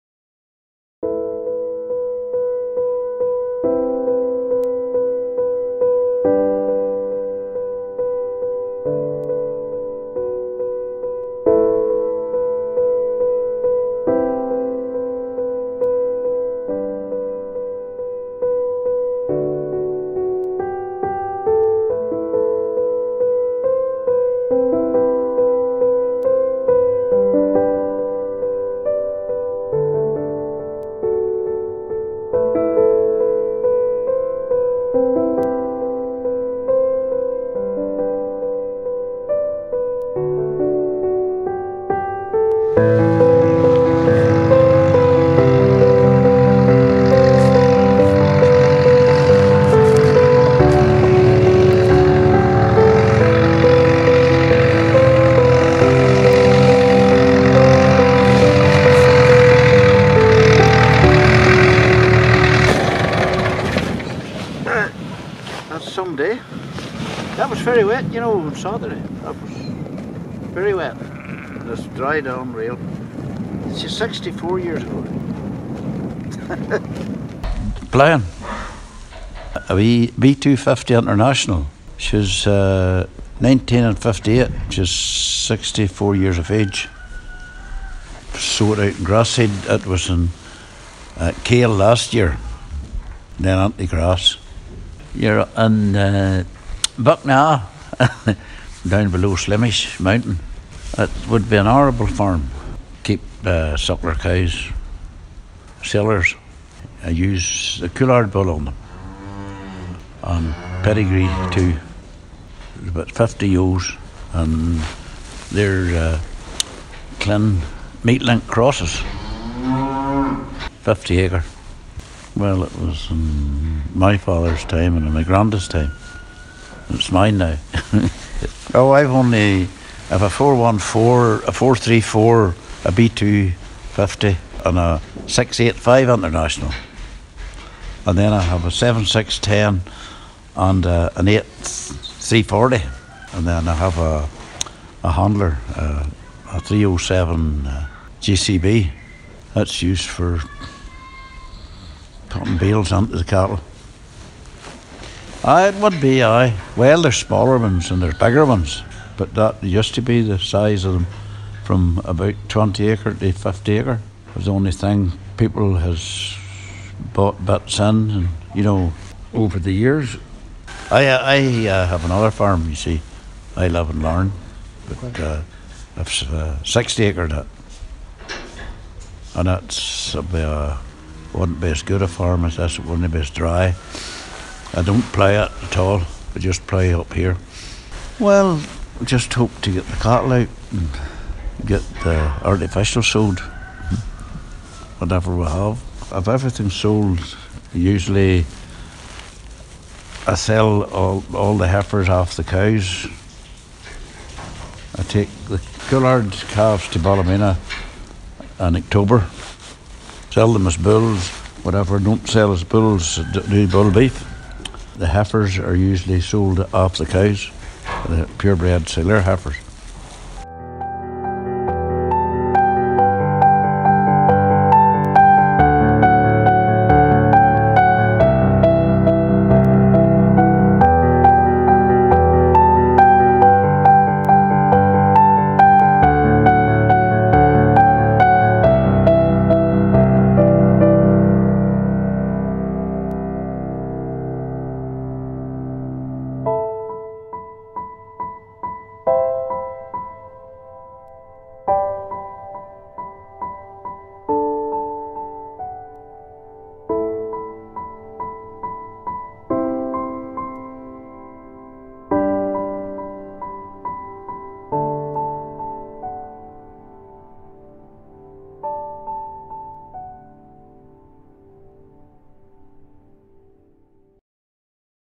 Robert Miller & B250 International Tractor At His Slemish Farm - (N Irish Farming Documentary0)
Robert is in his 80's but you would never know.   He's fit, has a great can-do attitude and a real love for his farms and vintage tractors.   This farm is in Buckna, below Slemish Mountain, County Antrim.   Robert is 3rd Generation on this land and he is a great grandfather, so the farm will always be in family hands.  The place has a real homely feel to it, and his son kindly give me a very informative tour of the land just as the new lambs are being born.
In this film Robert talks about tractors, crops, animals, farm sizes and what tasks are beginning in Spring.